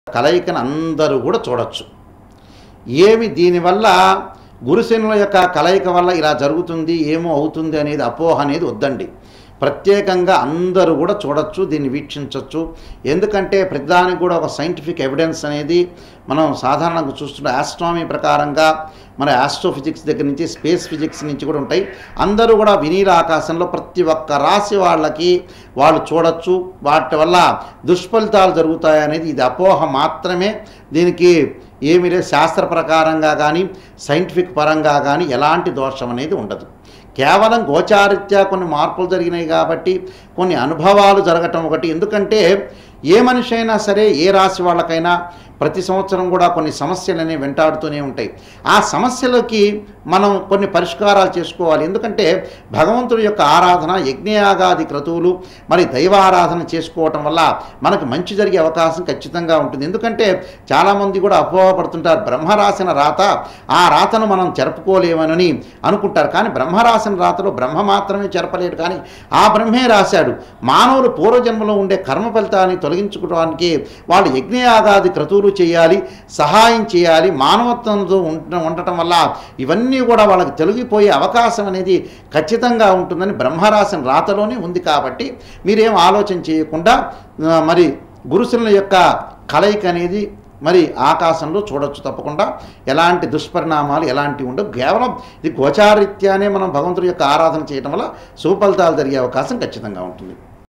గురు శని యొక్క కలయిక ఈ రాశి వారు చూడటం వలన ఏమి జరుగుతుందో తెలుసా ? | OM CVR
గురు శని గ్రహాల సంయోగం# వల్ల ప్రళయం తప్పదా ? | OM CVR
# గ్రహఫలం Astrology # CVROM
ఈ రోజు జరిగే ముఖ్య గ్రహమార్పులు అలాగే గ్రహస్థితిగతులలో కలిగే మార్పుల వలన ద్వాదశ రాశులపై ఎటువంటి ప్రభావాలు కలగబోతున్నాయో ఈ వీడియో లో ప్రముఖ జ్యోతిష్యులుశ్రీమాన్ వెంగళ పూర్ణాచార్య స్వామి గారు
తెలియ జేస్తున్నారు.